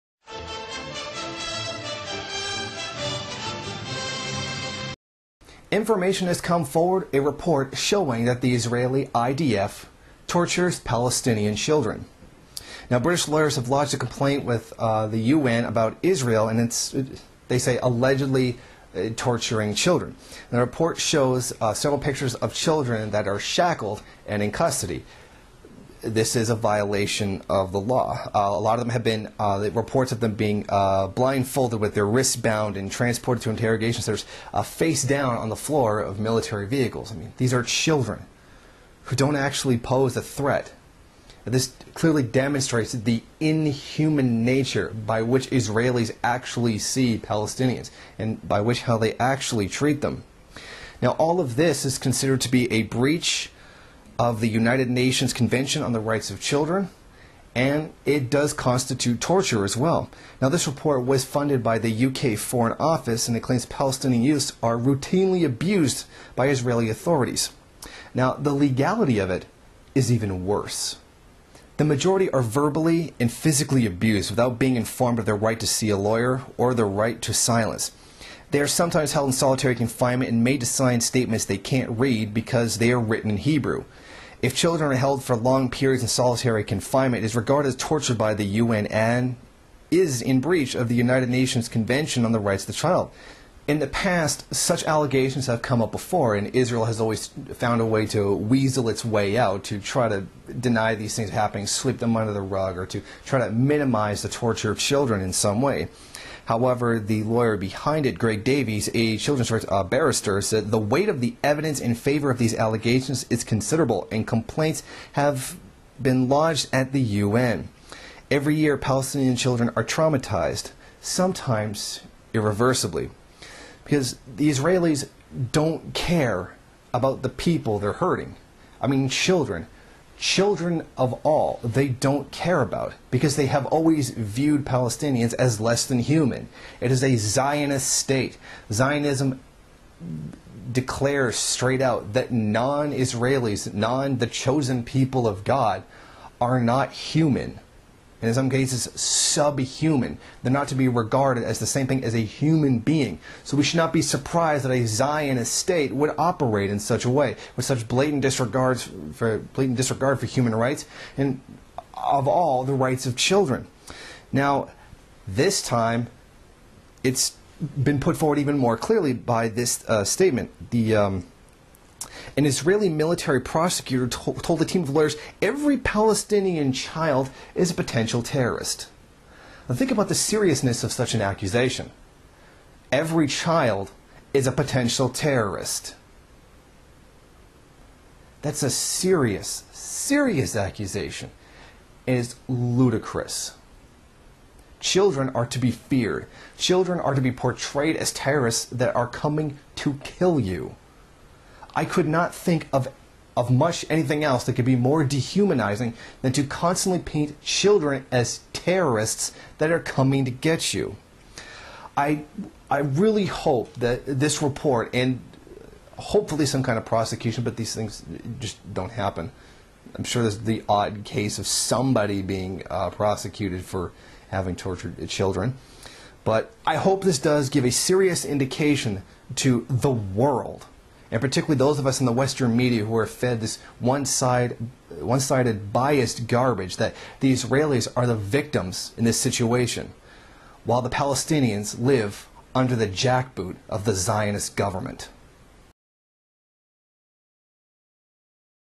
IDF Tortures Palestinian Children
The UK government has filed a lawsuit against the IDF for the abuse of Palestinian children.